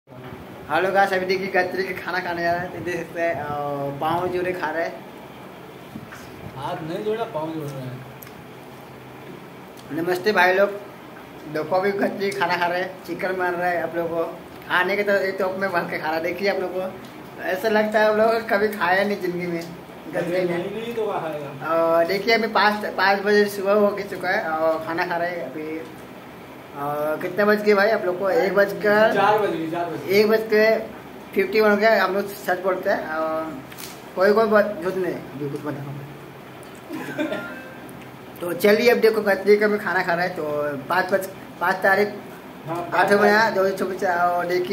5.8.2024 सोमवार सुबह chicken ka Gati banaa raha hai aap log De sakta hai bimar Mor se liya
5.8.2024 सोमवार सुबह chicken ka Gati banaa raha hai aap log De sakta hai bimar Mor se liya